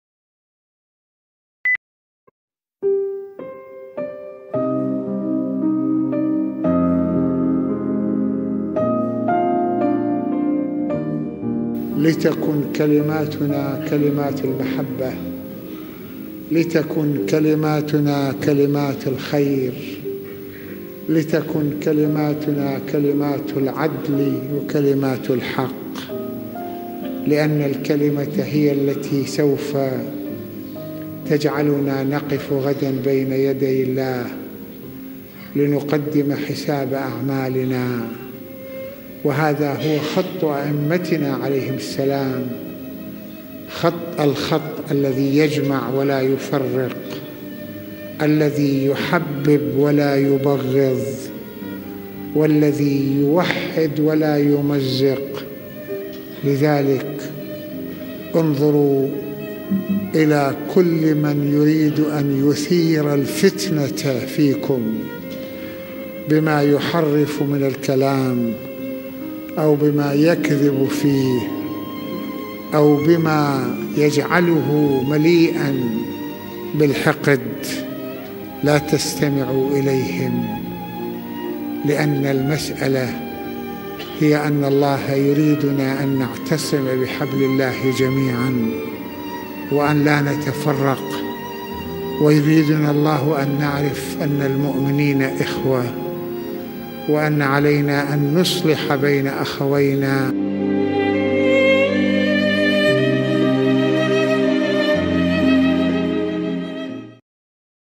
Pearls of Wisdom::Let our word be for good and loveكلمات للحياه:: لتكن كلمتنا كلمة للحبة و الخير
السيد محمد حسين فضل الله:: كلمات للحياة:: لتكن كلماتنا كلمات المحبة و الخير
Pearls of Wisdom:: Sayyed Mohamad Hussein Fadlallah:: English subtitles:: Let our words be for the good and for love